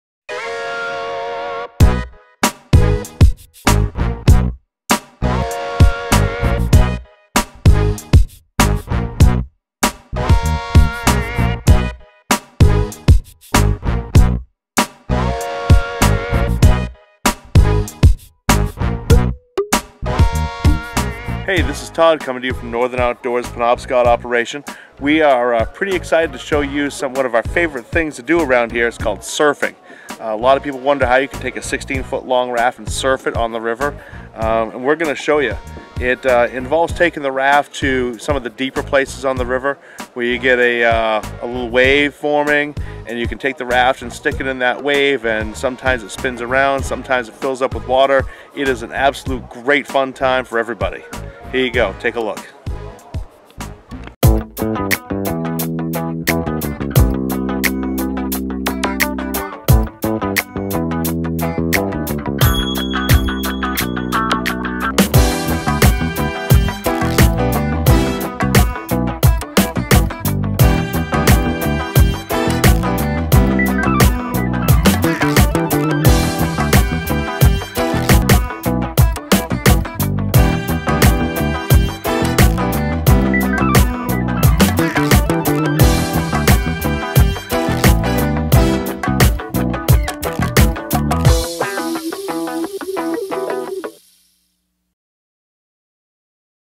Northern Outdoors: Maine Rafting- Penobscot River Surfing
Penobscot River rafting with Northern Outdoors is so much more than just a straight paddle down the rivah!! We stop and surf (yep, not quite like the Hawaiian surfers but same concept) in several places on the river that offer incredible fun and thrills. Surfing a 16 foot raft on the Penobscot is something for your bucket list of Maine adventures, for sure.  This video showcases some of our favorite spots on the river.  In addition to surfing, we paddle our rafts through gorgeous Ripgenous Gorge with breathtaking scenery, right past looming Mt Katahdin, the northern terminus of the famous Appalachian Trail in Baxter State Park, and stop for a ridiculously decadent hot grilled lunch along the riverbank mid way down the river. This is a Yankee Magazine best New England Adventures trip! The Penobscot runs from Memorial Day to the week after Labor Day - come on and join us.